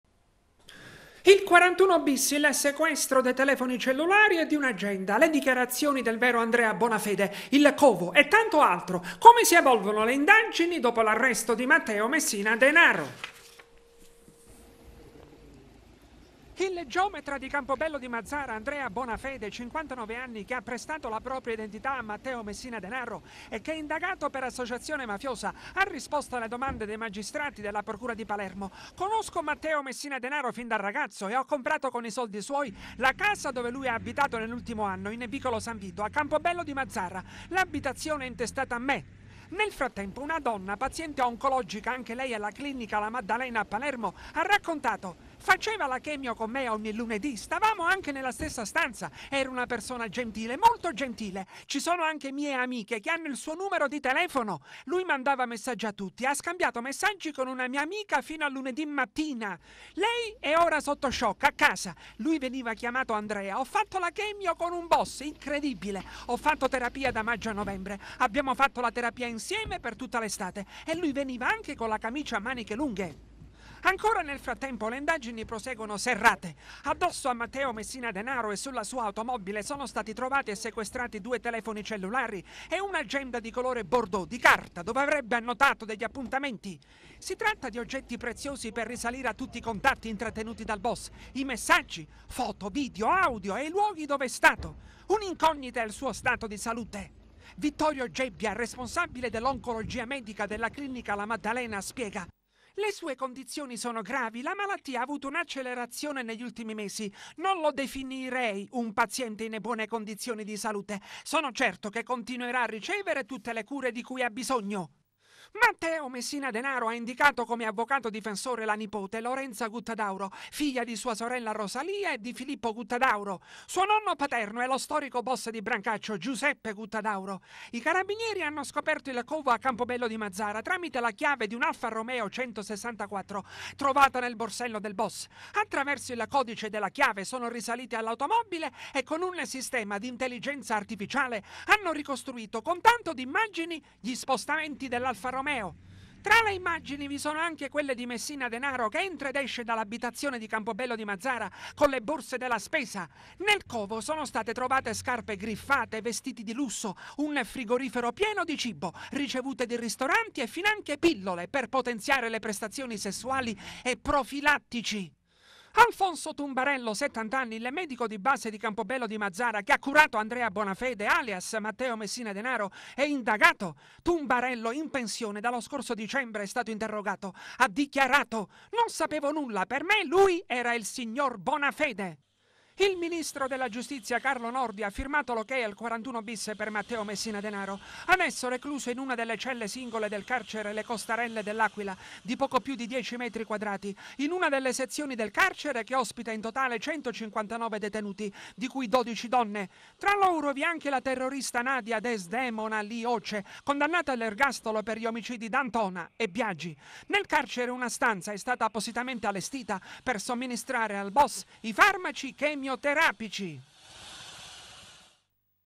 Ruoppolo Teleacras - “Messina Denaro”, l’evoluzione delle indagini
Il 41 bis, il sequestro dei telefoni cellulari e di un’agenda, le dichiarazioni del vero Andrea Bonafede, il covo e tanto altro: come si evolvono le indagini dopo l’arresto di Matteo Messina Denaro. Il servizio di Angelo Ruoppolo.

Il geometra di Campobello di Mazara, Andrea Bonafede, 59 anni, che ha prestato la propria identità a Matteo Messina Denaro, e che è indagato per associazione mafiosa, ha risposto alle domande dei magistrati della Procura di Palermo: “Conosco Matteo Messina Denaro fin da ragazzo, e ho comprato con i soldi suoi la casa dove lui ha abitato nell’ultimo anno, in vicolo San Vito, a Campobello di Mazara. L’abitazione è intestata a me”. Nel frattempo, una donna, paziente oncologica anche lei alla clinica “La Maddalena” a Palermo, ha raccontato: “Faceva la chemio con me ogni lunedì. Stavamo anche nella stessa stanza, era una persona gentile, molto gentile. Ci sono anche mie amiche che hanno il suo numero di telefono. Lui mandava messaggi a tutti. Ha scambiato messaggi con una mia amica fino a lunedì mattina. Lei è ora sotto shock a casa. Lui veniva chiamato Andrea. Ho fatto la chemio con un boss, incredibile. Ho fatto terapia da maggio a novembre. Abbiamo fatto la terapia insieme per tutta l’estate e lui veniva anche con la camicia a maniche lunghe”. Ancora nel frattempo le indagini proseguono serrate: addosso a Matteo Messina Denaro e sulla sua automobile sono stati trovati e sequestrati due telefoni cellulari e un’agenda di colore bordeaux, di carta, dove avrebbe annotato degli appuntamenti. Si tratta di oggetti preziosi per risalire a tutti i contatti intrattenuti dal boss, i messaggi, foto, video, audio, e i luoghi dove è stato. Un’incognita è il suo stato di salute. Vittorio Gebbia, responsabile dell’Oncologia medica della clinica “La Maddalena”, spiega: “Le sue condizioni sono gravi, la malattia ha avuto un’accelerazione negli ultimi mesi. Non lo definirei un paziente in buone condizioni di salute. Sono certo che continuerà a ricevere tutte le cure di cui ha bisogno”. Matteo Messina Denaro ha indicato come avvocato difensore la nipote, Lorenza Guttadauro, figlia di sua sorella Rosalia e di Filippo Guttadauro. Suo nonno paterno è lo storico boss di Brancaccio, Giuseppe Guttadauro. I Carabinieri hanno scoperto il covo a Campobello di Mazara tramite la chiave di un’Alfa Romeo 164 trovata nel borsello del boss. Attraverso il codice della chiave sono risaliti all’automobile e, con un sistema di intelligenza artificiale, hanno ricostruito, con tanto di immagini, gli spostamenti dell’Alfa Romeo. Tra le immagini vi sono anche quelle di Messina Denaro che entra ed esce dall’abitazione di Campobello di Mazara con le borse della spesa. Nel covo sono state trovate scarpe griffate, vestiti di lusso, un frigorifero pieno di cibo, ricevute di ristoranti, e finanche pillole per potenziare le prestazioni sessuali e profilattici. Alfonso Tumbarello, 70 anni, il medico di base di Campobello di Mazara, che ha curato Andrea Bonafede, alias Matteo Messina Denaro, è indagato. Tumbarello, in pensione dallo scorso dicembre, è stato interrogato. Ha dichiarato: “Non sapevo nulla. Per me lui era il signor Bonafede”. Il ministro della Giustizia, Carlo Nordio, ha firmato l’ok al 41bis per Matteo Messina Denaro, adesso recluso in una delle celle singole del carcere “Le Costarelle” dell’Aquila, di poco più di 10 metri quadrati, in una delle sezioni del carcere che ospita in totale 159 detenuti, di cui 12 donne. Tra loro vi è anche la terrorista Nadia Desdemona Lioce, condannata all’ergastolo per gli omicidi D’Antona e Biagi. Nel carcere una stanza è stata appositamente allestita per somministrare al boss i farmaci chemioterapici.